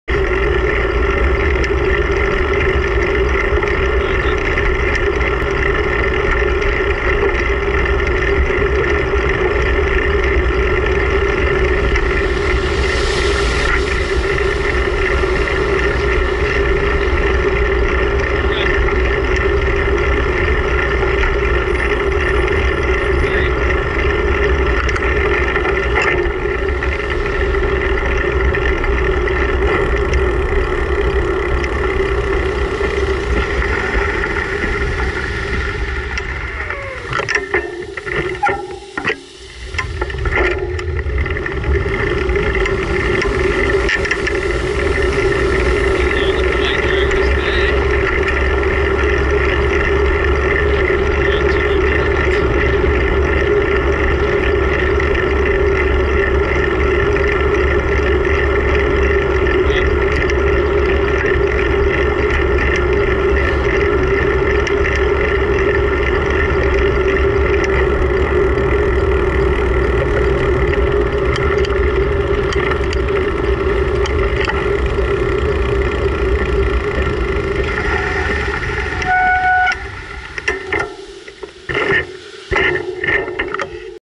Santa Isabel in Costa Mesa: close pass and failure to yield at stop sign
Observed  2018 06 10